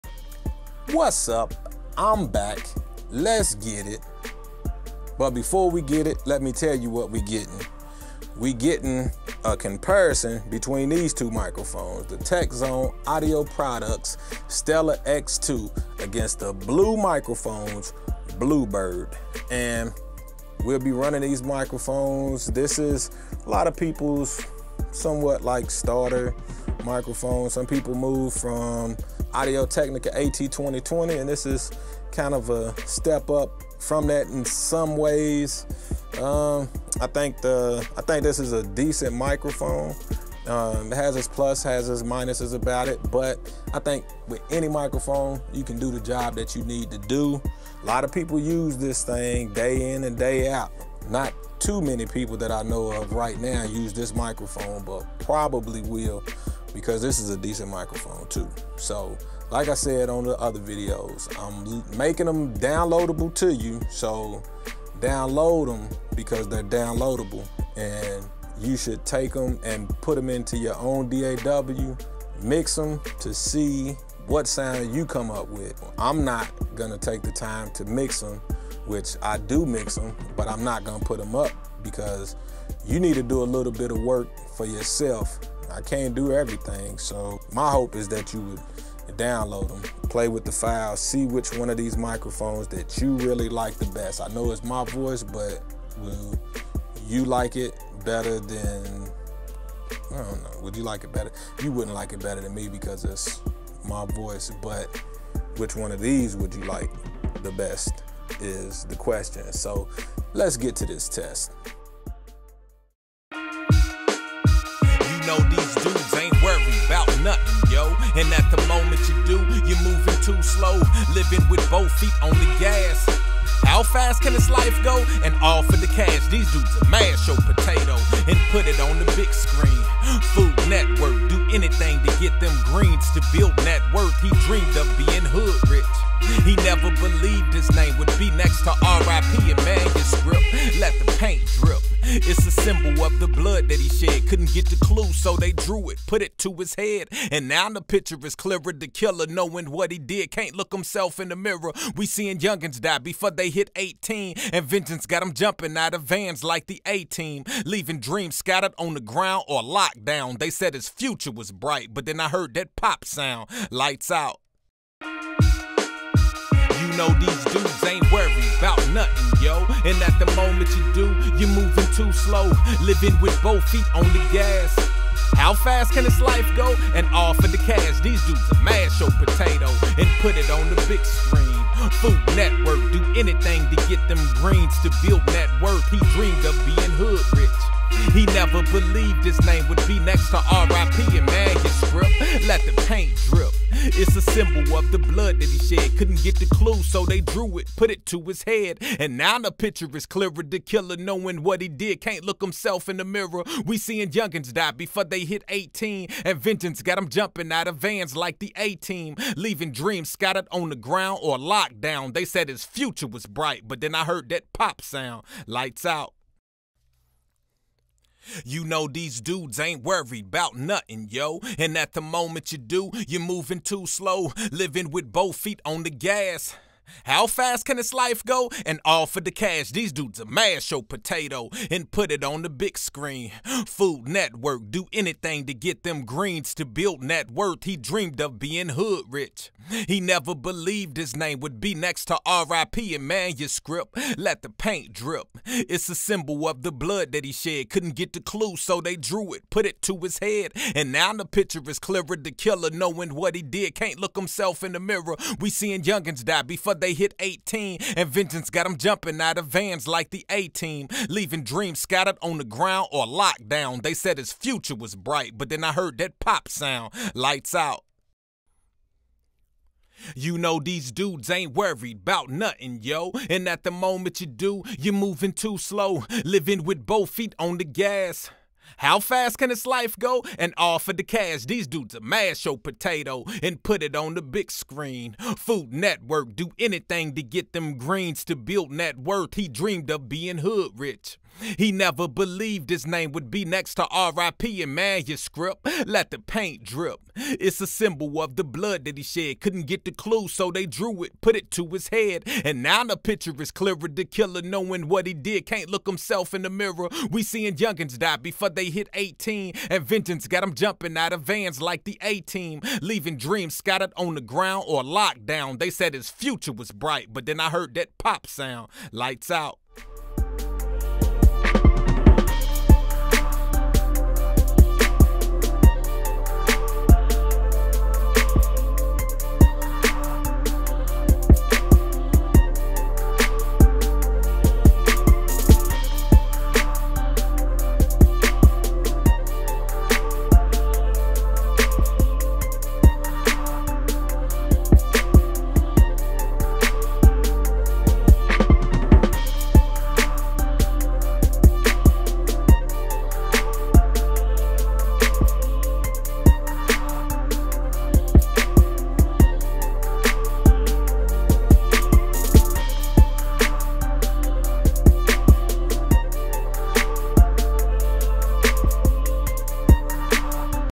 Stellar X2 vs Bluebird (Apollo Neve 1073)
In this video I compare the Stellar X2 with the Blue Microphones’s Bluebird using the Universal Audio Apollo’s Neve 1073 unison preamp. This is a blind test with the answer revealed at the end of the video. The first takes have music playing halfway through. The second takes are vocal only. These files are downloadable so that you can hear the full quality and so that you can mix them in your own DAW.

Instagram.com/cleanmudvideos